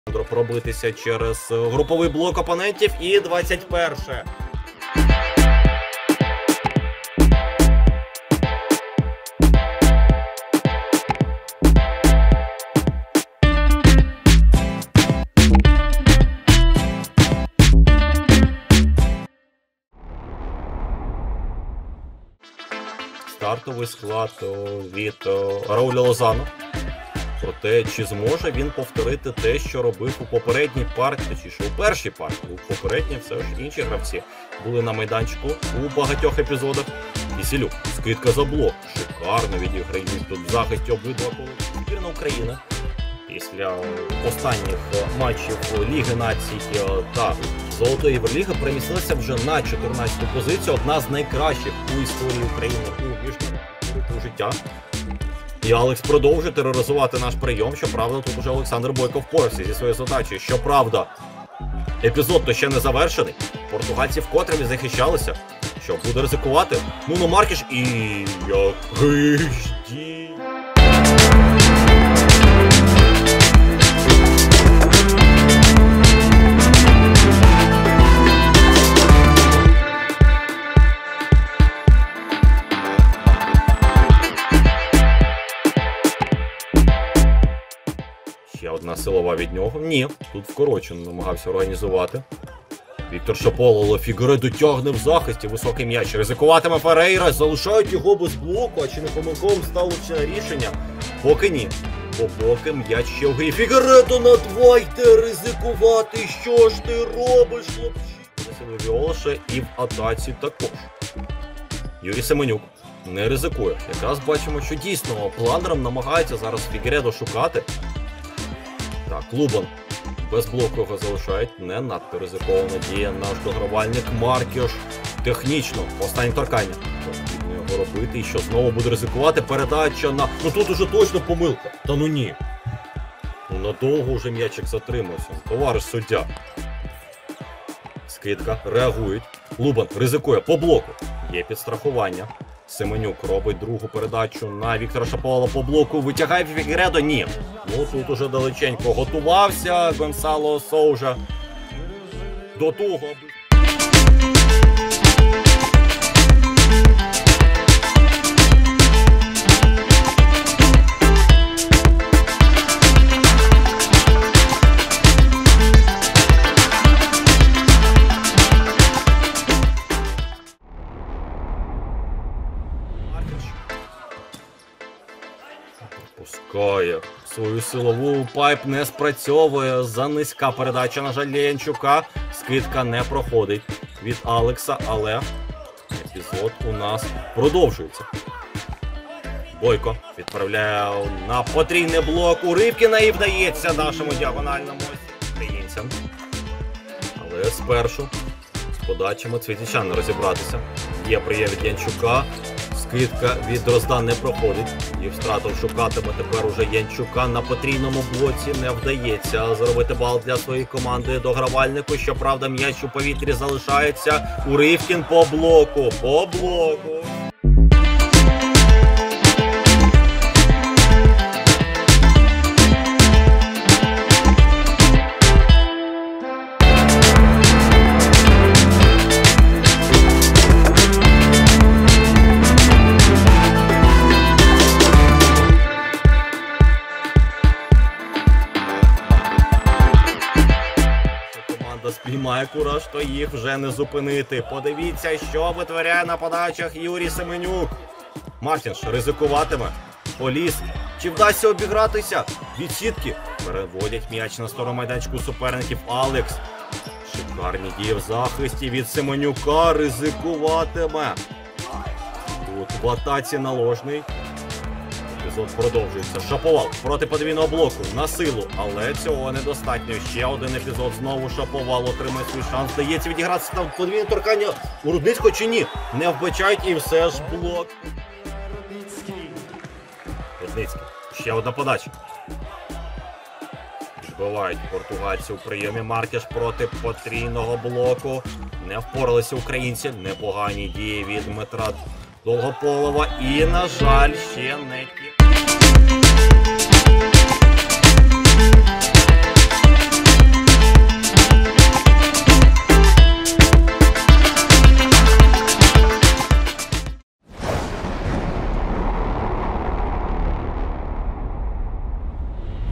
Португалія - Україна | ТОП-розіграшів | Волейбол | CEV European Golden League 2024 | Жінки
Емоційною та напрочуд цікавою видалася гра збірних Португалії та України. Ще б пак!
Запальні португальці та не менш азартні українці - це протистояння стало і волейбольним дійством і справжнім виром емоцій. І нехай хлопці із Португалії потішили жіночі очі, проте наші козаки порадували усіх, хто вболіває за український волейбол. Віддали один сет опонентам, проте звитягу записали на власний рахунок, таким чином забезпечивши собі достроковий вихід у фінал чотирьох Золотої Євроліги.
Вітаємо підопічних Рауля Лосано і переглядаємо класні моменти цього поєдинку.